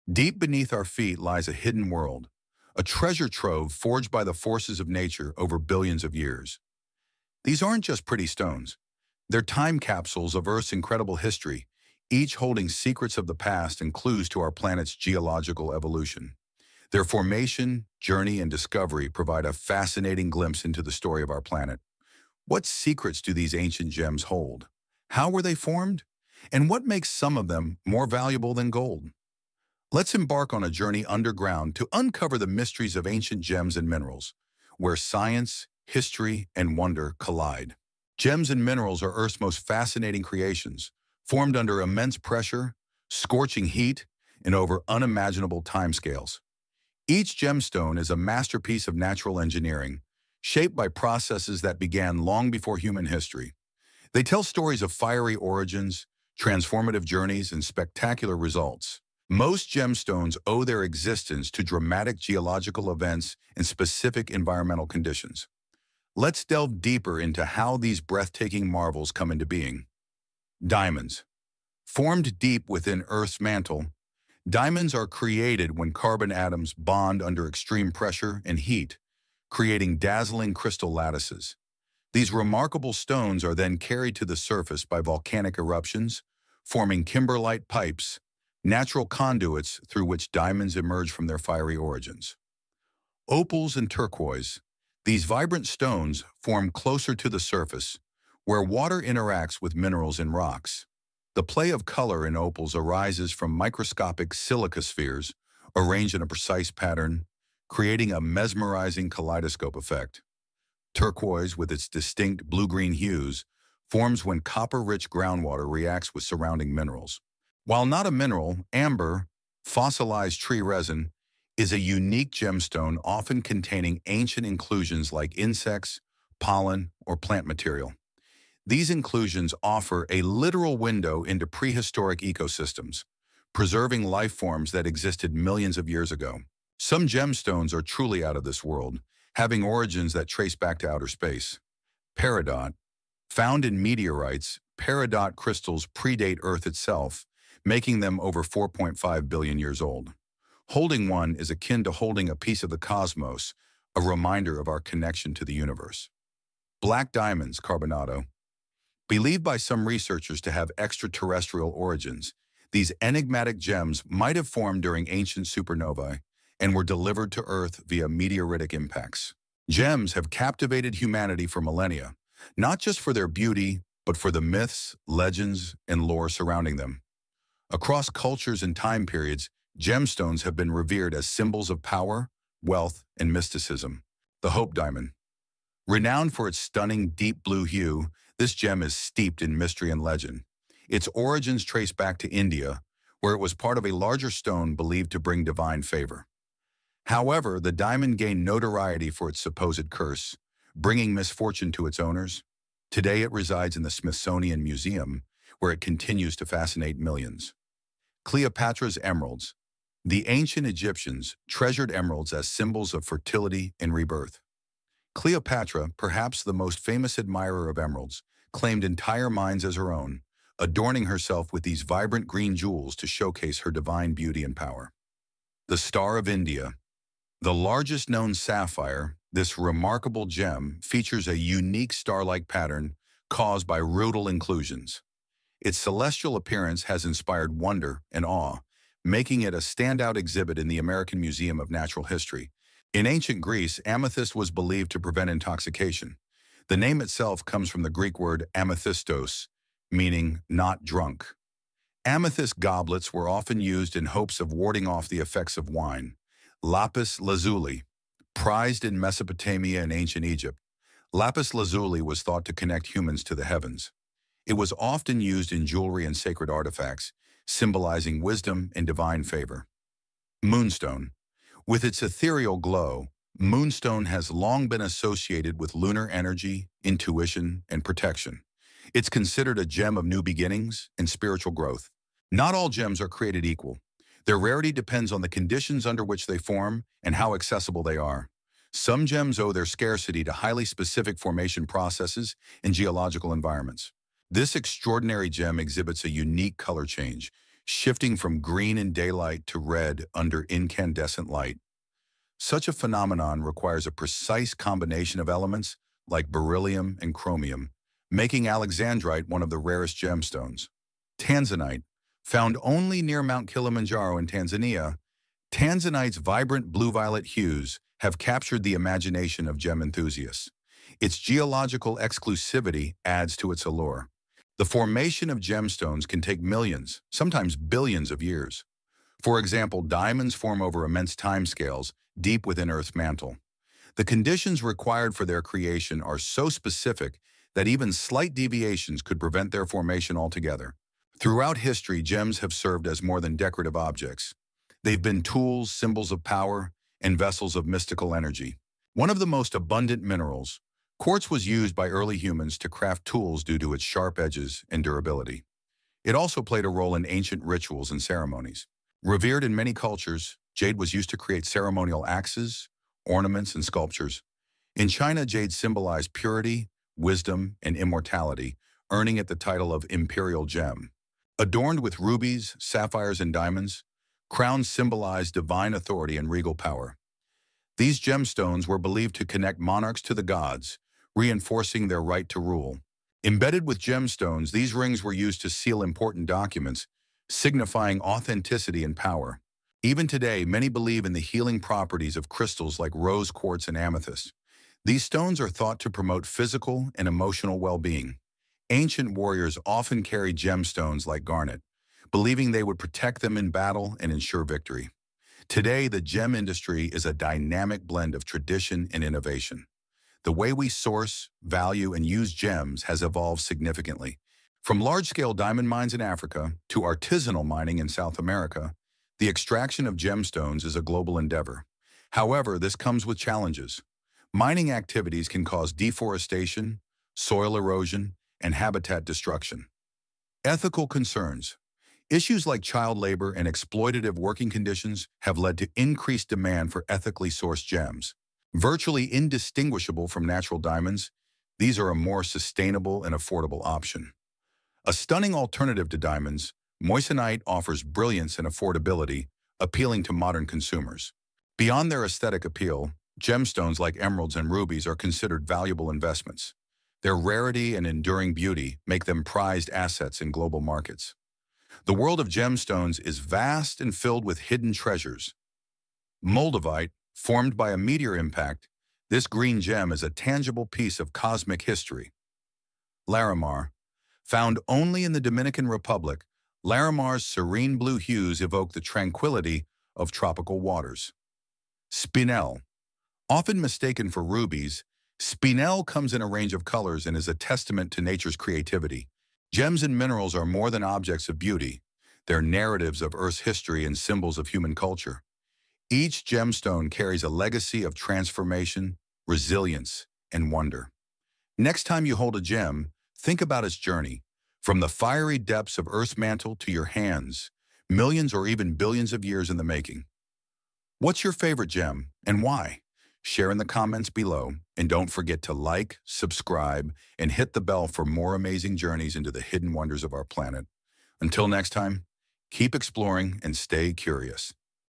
Ancient Gems and Minerals - Treasures Hidden Underground
Discover the hidden wonders beneath the Earth's surface! 🌍✨ Explore the formation, myths, and mysteries of ancient gems and minerals.

From diamonds forged in Earth's mantle to cosmic peridot older than the planet itself, each gem tells a story billions of years in the making. Uncover their legendary tales, scientific marvels, and cultural significance in this fascinating journey into the world of Earth's sparkling treasures.